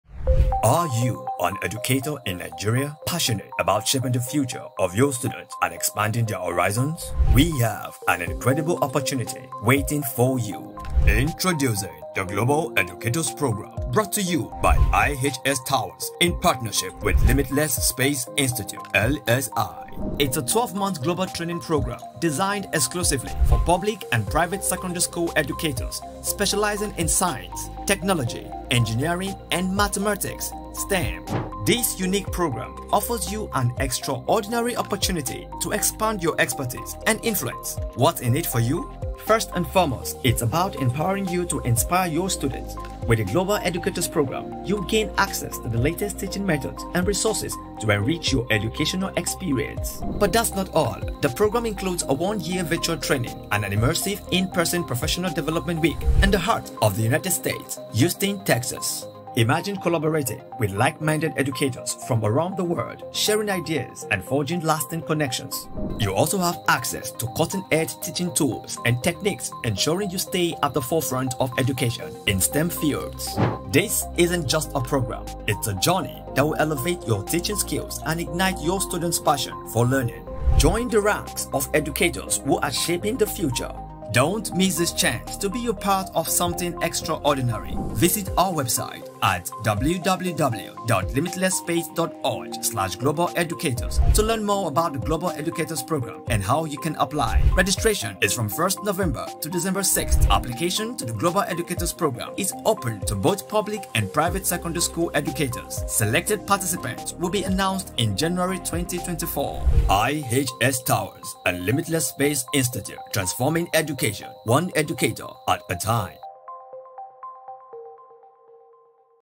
IHS Advert
Attention all dedicated educators in the fields of Science, Technology, Engineering, and Mathematics —this one's for you!

During this intensive virtual year-long program, participants will delve into Space Science, Sustainability, Climate Change, and Space Ethics. Plus, here's the exciting part: they’ll  experience an In-Person Professional Development Week in Houston, Texas, United States of America, the epicenter of human space exploration.

The registration and entry periods are open from November 1 to December 6, 2023, selected participants will be announced in January 2024.

Please note that this opportunity is exclusively for Public and Private Secondary School Level Educators.